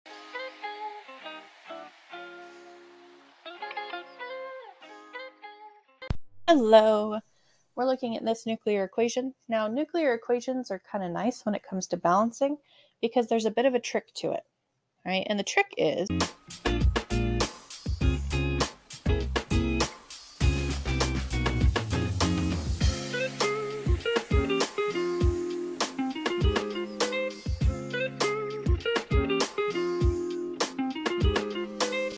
(10.2) How many neutrons are generated in the nuclear equation shown below? Xe + 249Pcm â†’ 37Rf + …
(10.2) How many neutrons are generated in the nuclear equation shown below? Xe + 249Pcm â†’ 37Rf + 1n Select one: a. 3 b. 2 c. 1 d. 0 Check (10.2) Which species completes this nuclear equation shown below?